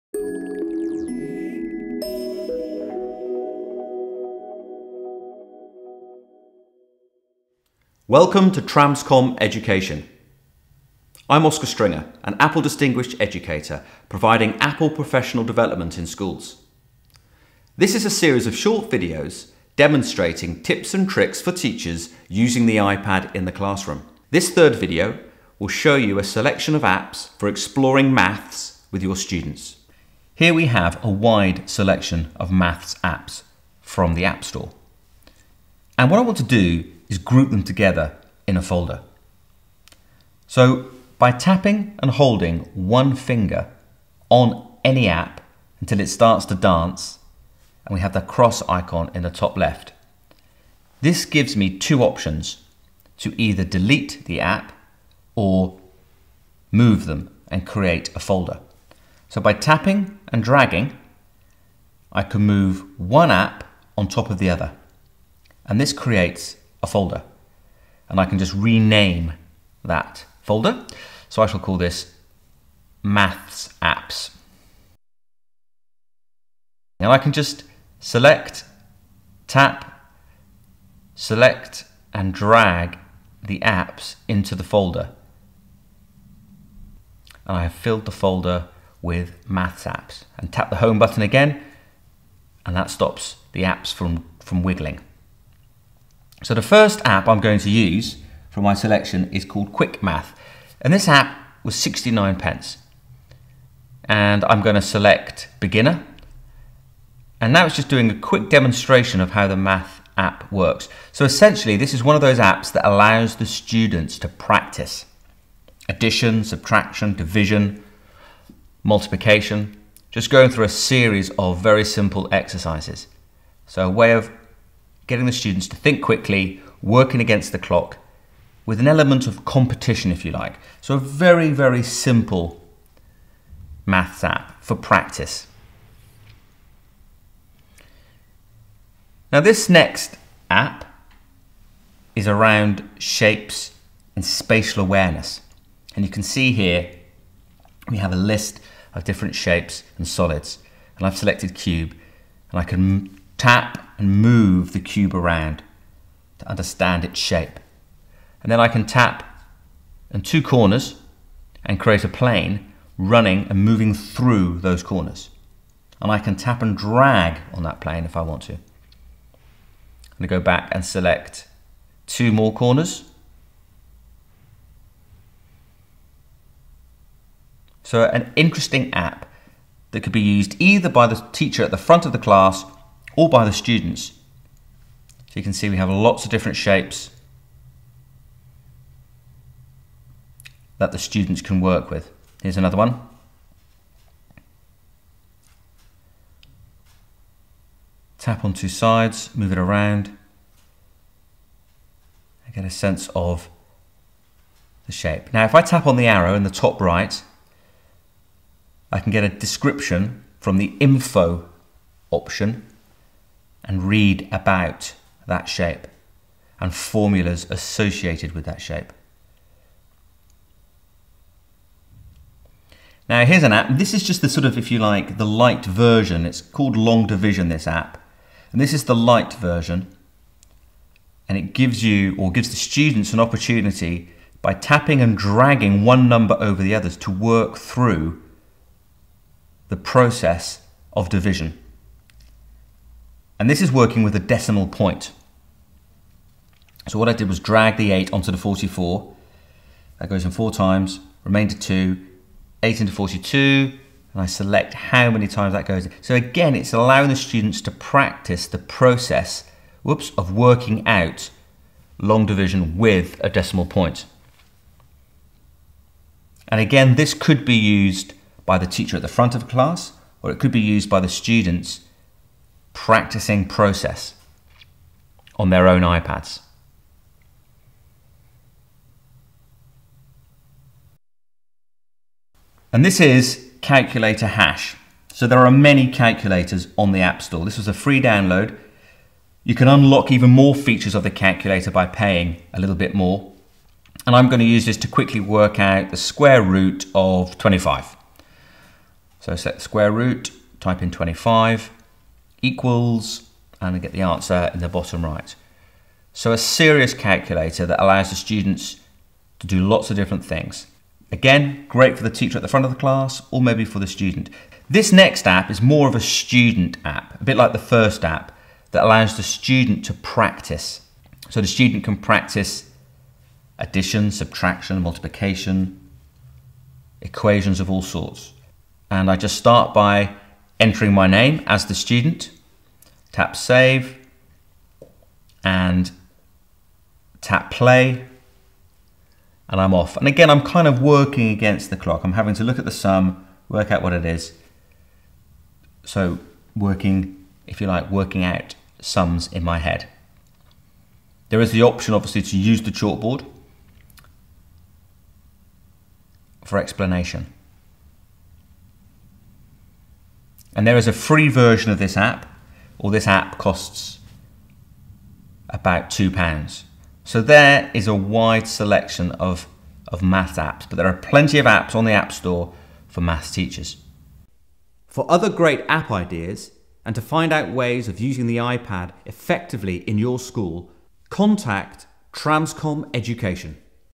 Tramscom Education - Maths Apps for iPad
In this video we show you a selection of iPad Apps for exploring Maths with students.